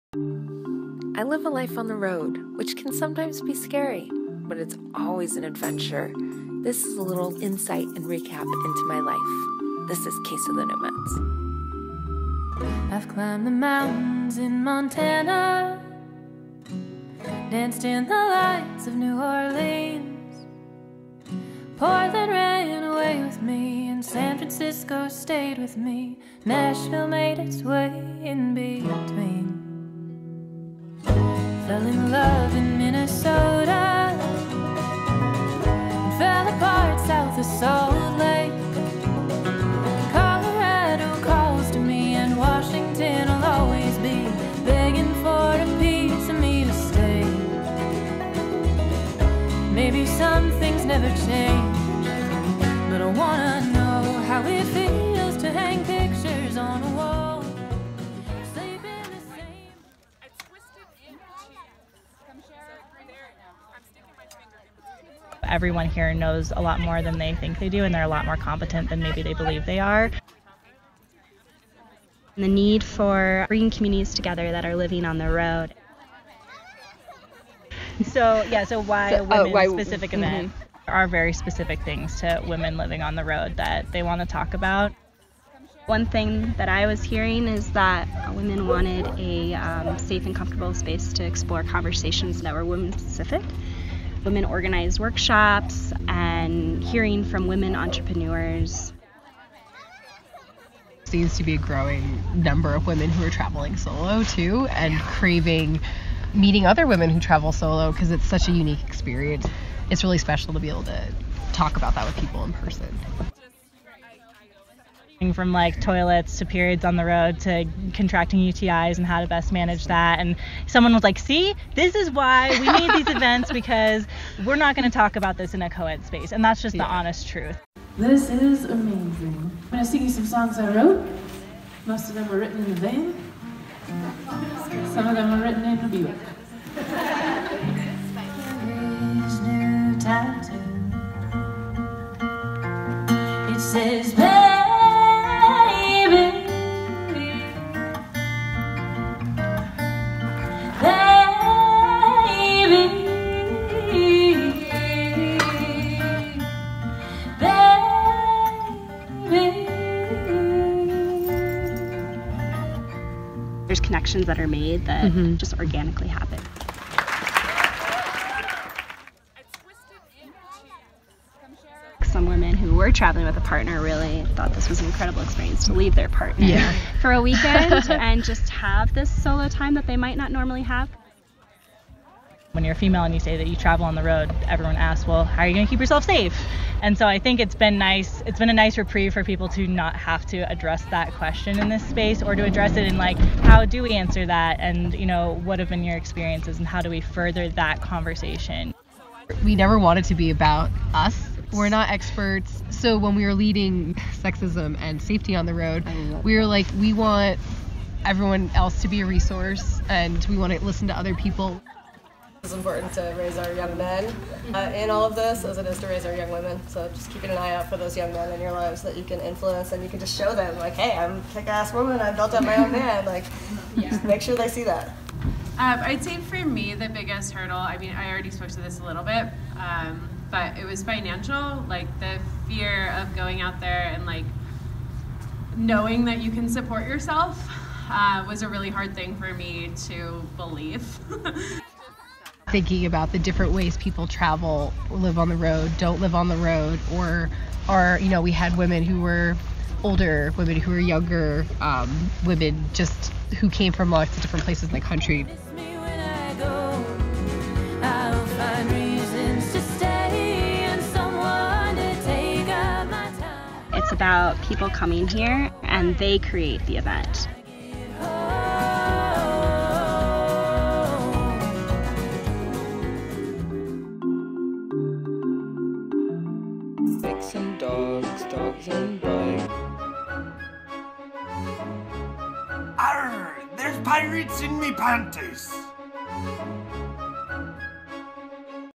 COTN Episode: Women on the Road Gathering
A film short about the first Women on the Road Gathering that took place in Taos, New Mexico in 2018. Featuring music from Ira Wolf and interviews with gathering organizers: Gale Straub from She-Explores, Laura Hughes from Women on the Road Podcast, and Kathleen Morton from Vanlife Dairies.